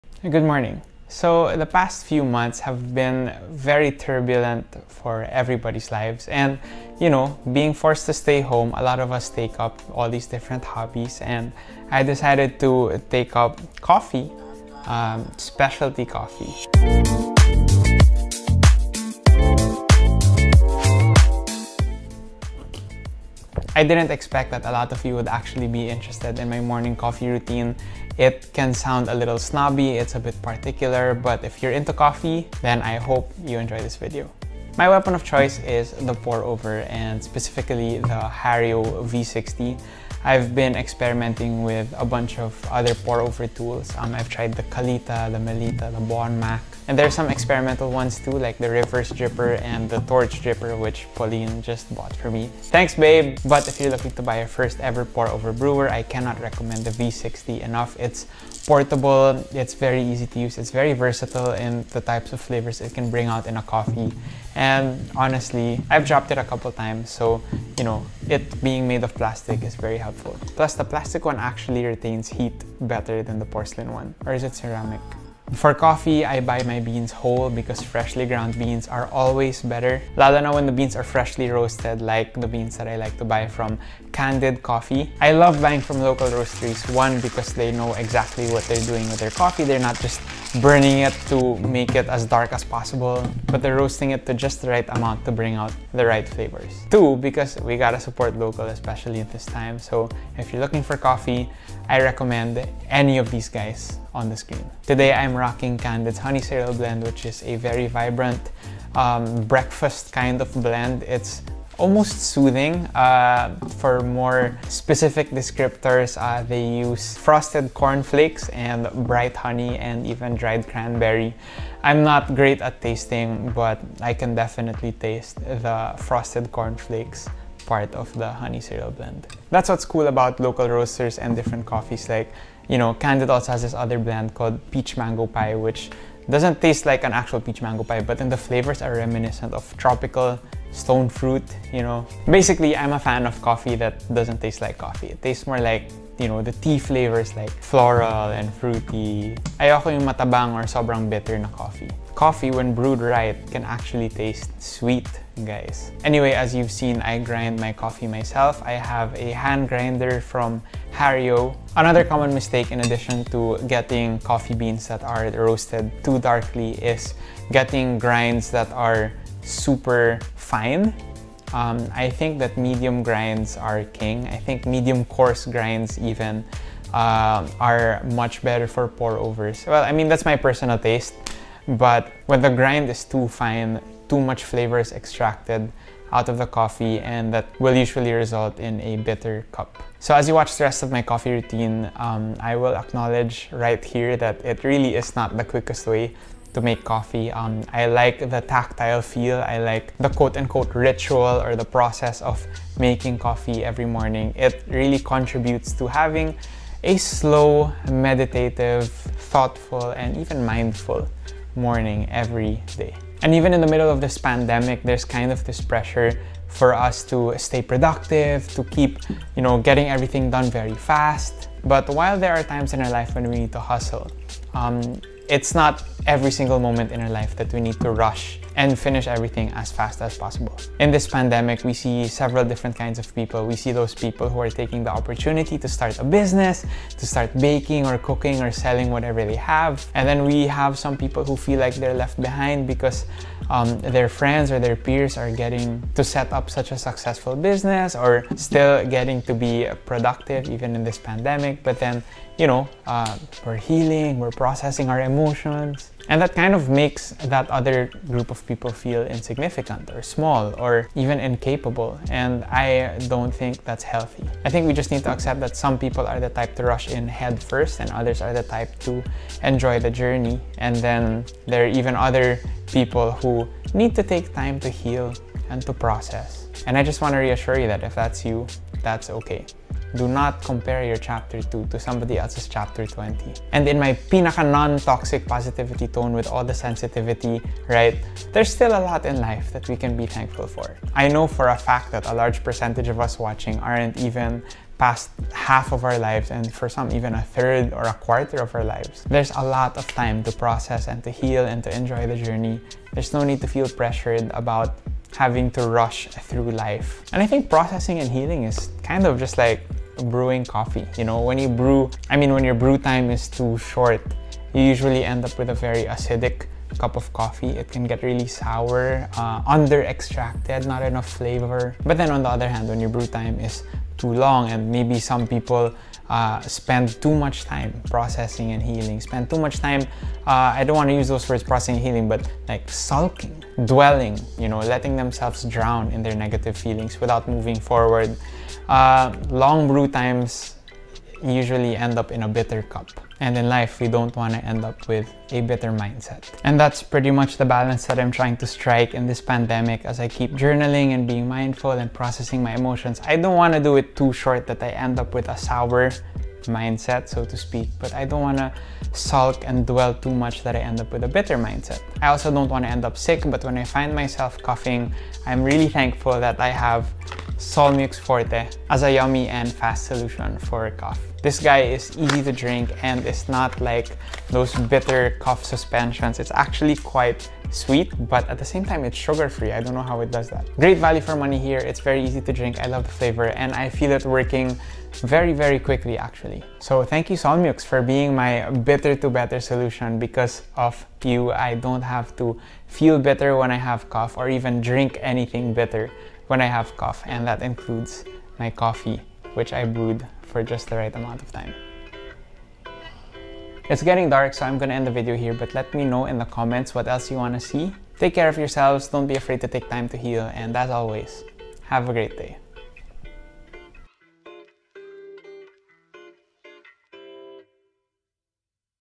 How I Brew Coffee That Isn't Bitter (V60 Pour Over)
📒 Learning to brew great specialty coffee by hand is one of the most rewarding skills I've learned this quarantine. There's a HUGE difference between putting "a few scoops" of pre-ground (and finely ground) coffee into a French press for "a few minutes", versus grinding a precise amount of coffee beans yourself and pouring hot water onto a V60, gram by gram. ☕

I don't mean to be a snob. 😅 But whether you're cooking or baking or brewing, being intentional and purposeful with the ingredients and process will always produce a better (and not bitter!) result overall. 🎂

So let's be purposeful with our lives—our morning habits, our words towards loved ones, the content we consume, our online shopping (!), how we spend our evenings, and the motives behind our daily living. 🙏